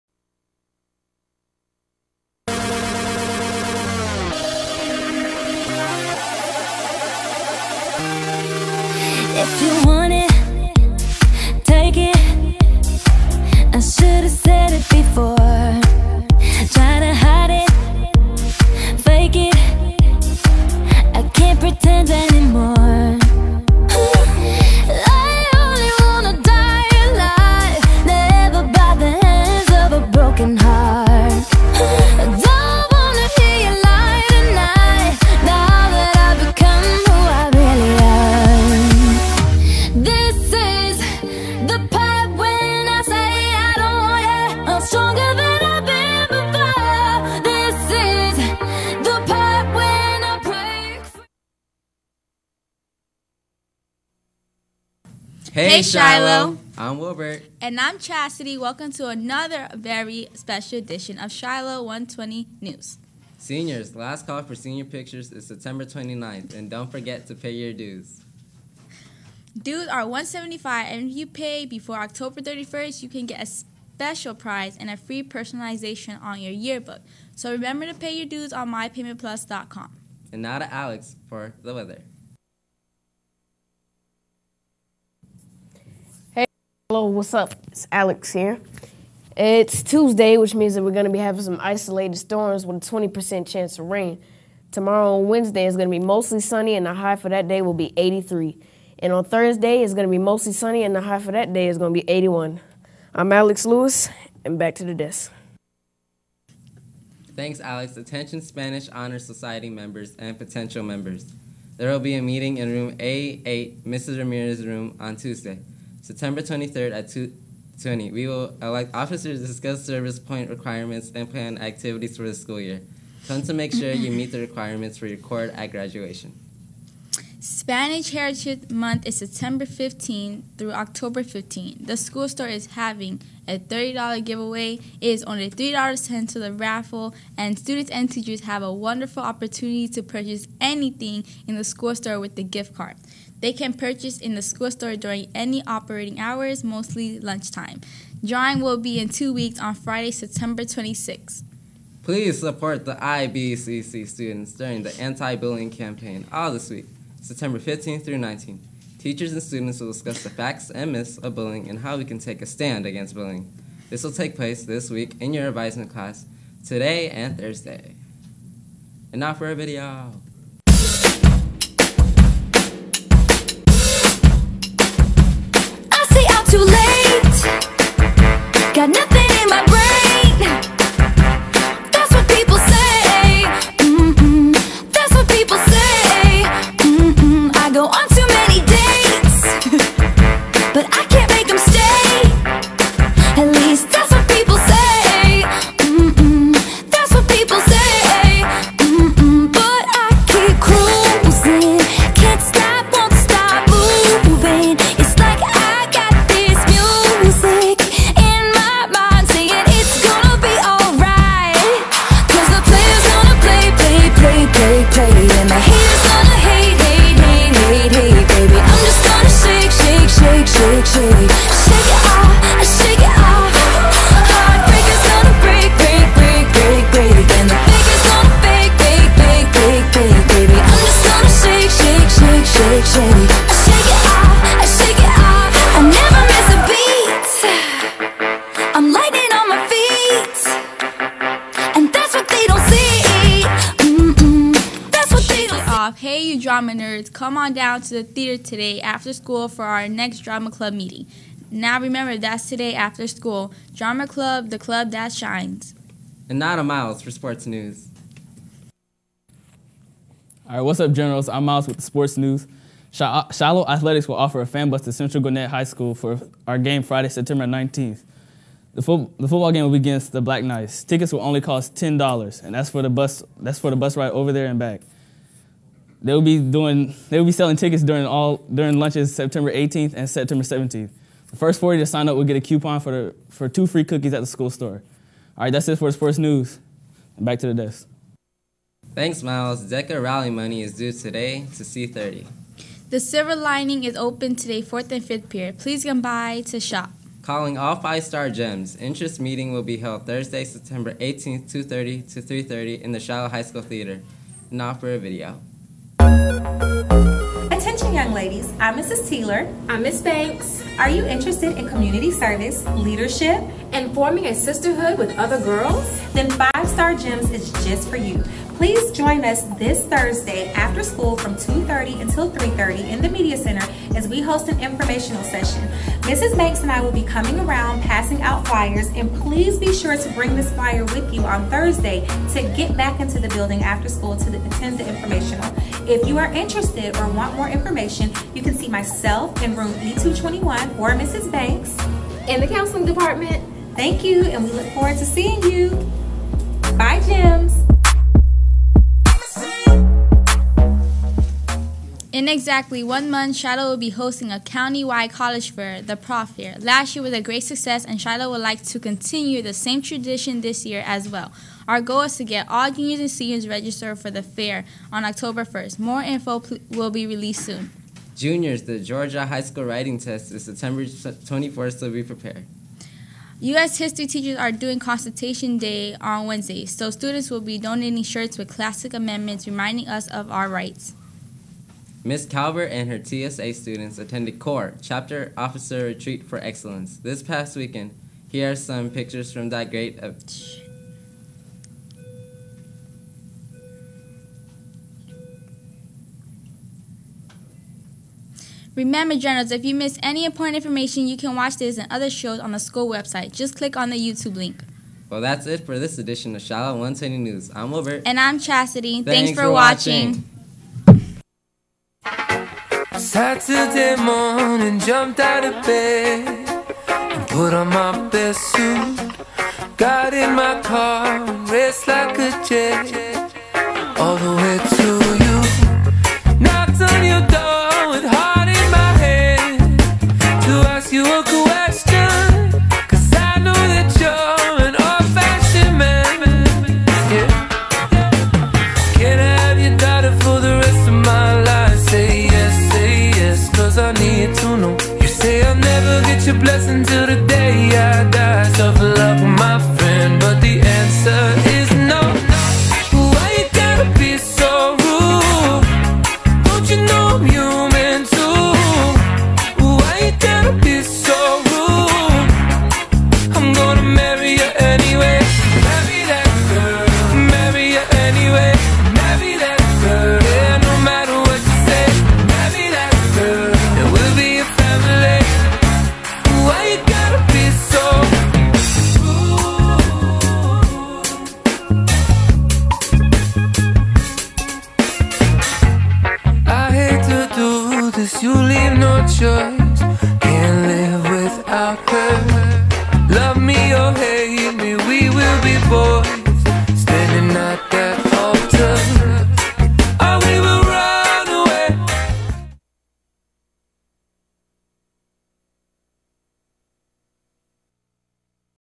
The Shiloh High School 120 News Show
Broadcast from Shiloh High School and replayed at 10:21, 10:51, 11:21 and 11:51 on Tuesday, Thursday and Fridays on Channel 2 of our school broadcast feed.